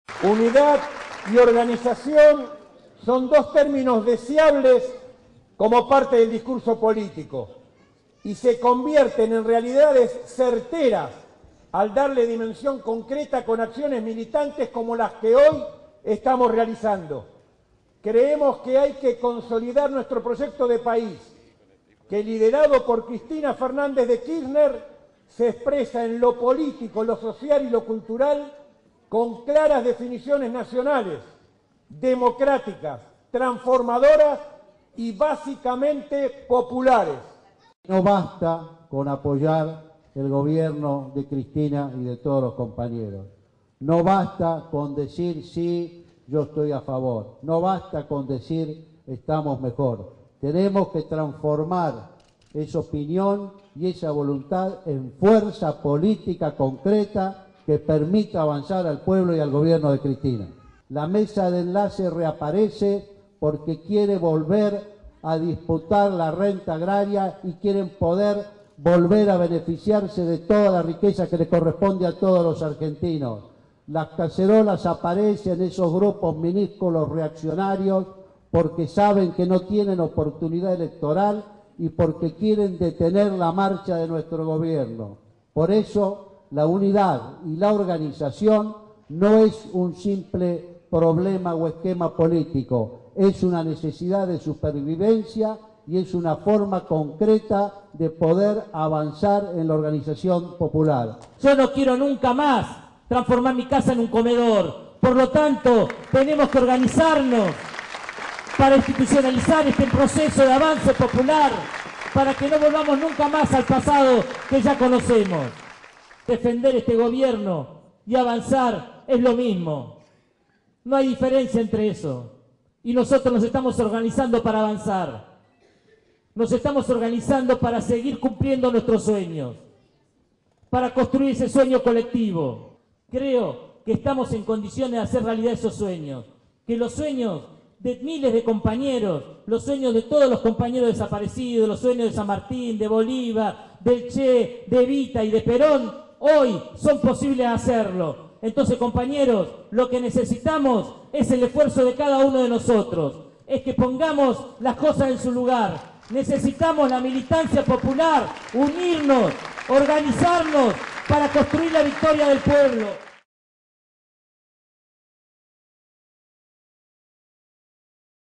La Corriente Peronista Federal se sumó al Movimiento Evita.
En respuesta a la consigna "Unidos y Organizados" que lanzó la Presidenta Cristina Fernández de Kirchner, el pasado 27 de abril en el multitudinario acto en Vélez, la Corriente Peronista Federal (CPF) se integró al Movimiento Evita.

Entre los oradores que participaron el acto realizado en el porteño Teatro Empire, se destacaron el ex canciller Jorge Taiana; el líder del Movimiento Evita, Emilio Pérsico; y el coordinador general de la TV Digital, Osvaldo Nemirovsci.

"Defender este gobierno y avanzar es lo mismo", expresó Pérsico, quien manifestó que no quiere "nunca más" transformar su casa "en un comedor".

Por su parte, Taiana advirtió sobre las verdaderas intenciones de las protestas de la Mesa de Enlace y algunos grupos minoritarios que organizaron "cacerolazos" en algunos barrios porteños.

"Las cacerolas aparecen porque saben que no tienen oportunidad electoral y porque quieren detener la marcha de nuestro gobierno", dijo.

La CPF está integrada por dirigentes históricos de las 24 provincias, entre ellos legisladores nacionales, provinciales, intendentes, dirigentes sociales y políticos.